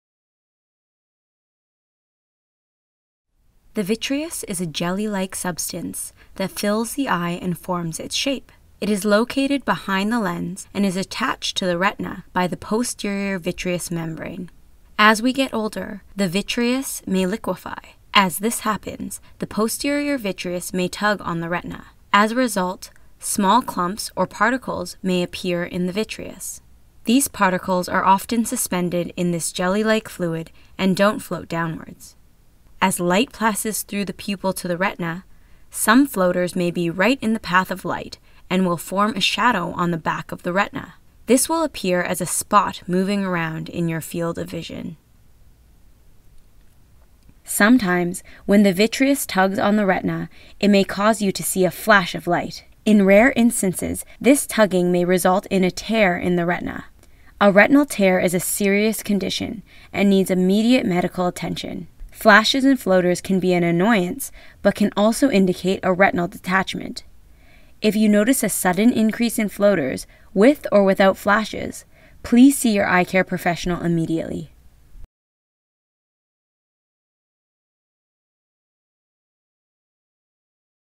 Flashes & Floaters
The vitreous is a jelly like substance that fills the eye and forms its shape. It is located behind the lens and is attached to the retina by the posterior vitreous membrane. As we get older, the vitreous may liquify.  As this happens, the posterior vitreous may tug on the retina. As a result, small clumps or particles may appear in the vitreous. These particles are often suspended in this jelly like fluid and don’t float downward. As light passes through the pupil to the retina, some floaters may be right in the path of light and will form a shadow on the back of the retina. This will appear as a spot moving around in your field of vision.

Sometimes when the vitreous tugs on the retina, it may cause you to see a flash of light. In rare incidences, this tugging may result in a tear in the retina.  A retinal tear is a serious condition and needs immediate medical attention. Flashes and floaters can be an annoyance, but can also indicate a retinal detachment. If you notice a sudden increase in floaters with our without flashes, please see your eye care professional immediately.

Visit an eye care professional to learn more.